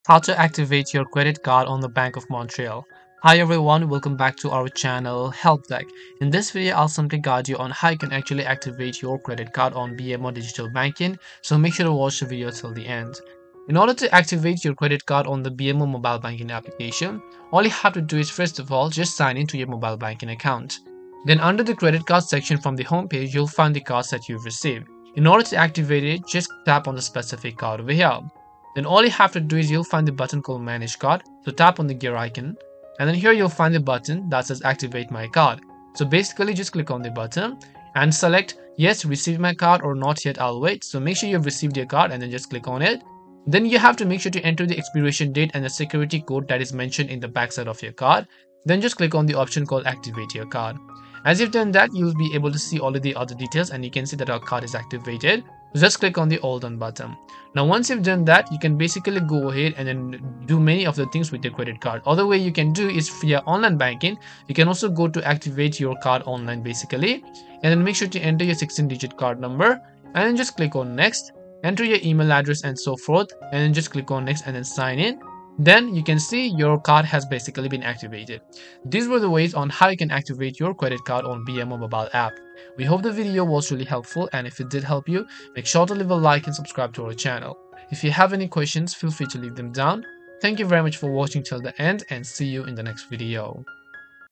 BMO Bank - How to Activate Credit Card - Bank of Montreal
This video guides you in quick easy steps to activate your credit card from the BMO app. So make sure to watch this video till the end.

~ Chapters:
0:00 Introduction
0:14 Sign In
0:21 Activate Credit Card
1:19 Conclusion